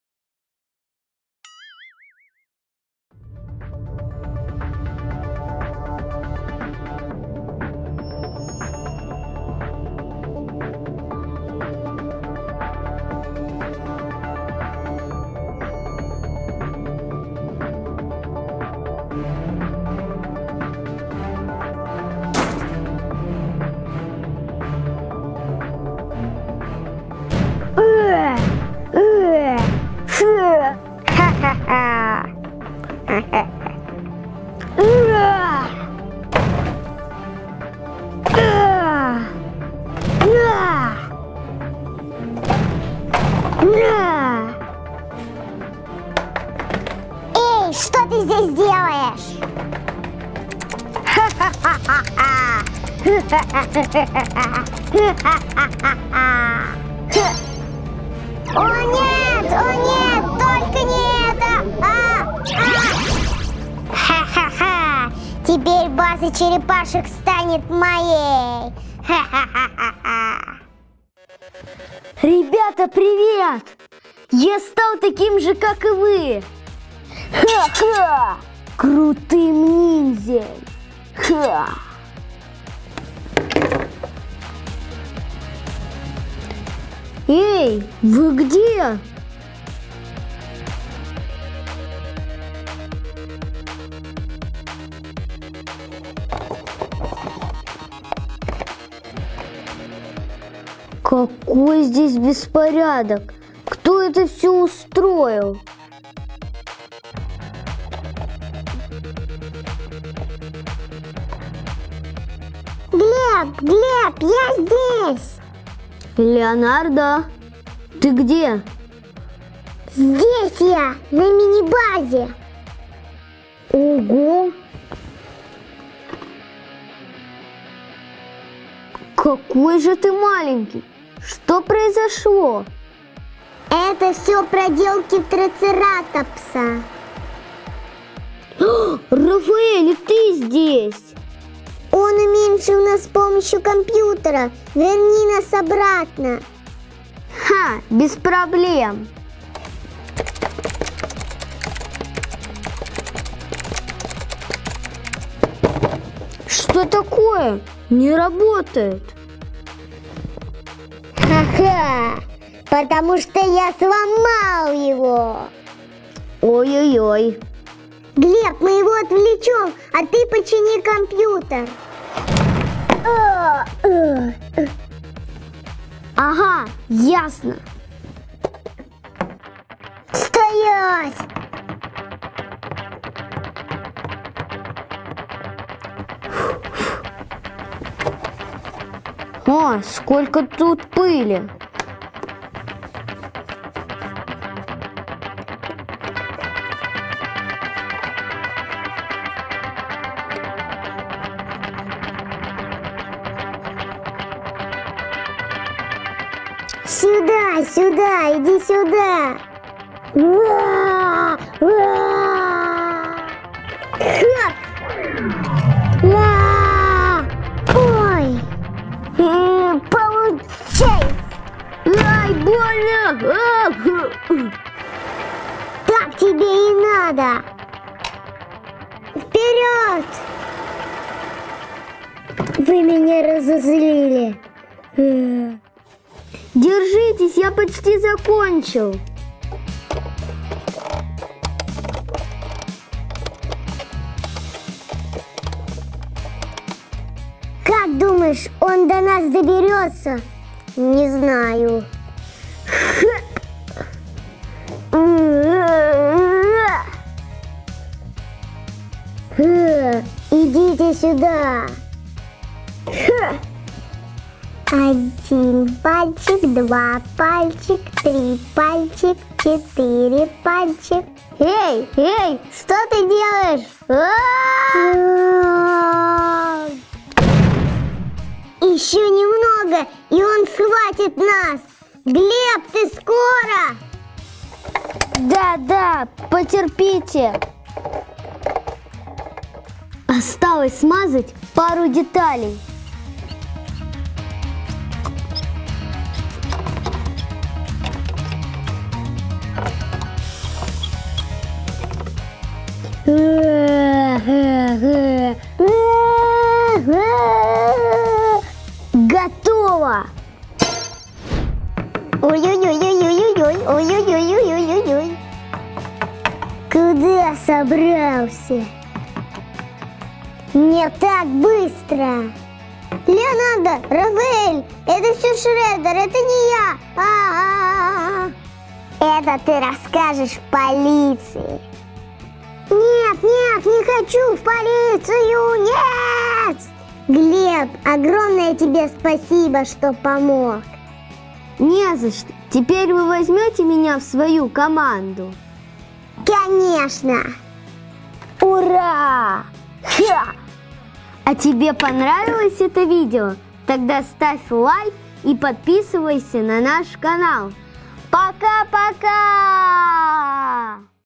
Черепашки Ниндзя. Крутые игровые наборы от Гулливер
Смотри новое видео с черепашками ниндзя и игровыми наборами Gulliver. У Глеба есть крутые игровые наборы с фигурками Черепашки Ниндзя. Сегодня Глебу и черепашкам предстоит сразиться с трицератопсом, который уменьшил Леонардо и Рафаэлло с помощью компьютера. Трицератопс решил захватить базу черепашек. Но не тут-то было. Глеб и храбрые игрушки черепашки справятся с ним!

- Все фигурки из мультика про черепашек тут
- Набор "Обсерватория Крэнга" из Черепашек-ниндзя с фигурками Крэнга и Кейси Джонса
- Фургон TMNT Черепашек с фигуркой.

Интерактивные фигурки, роботы, машинки и любимые герои.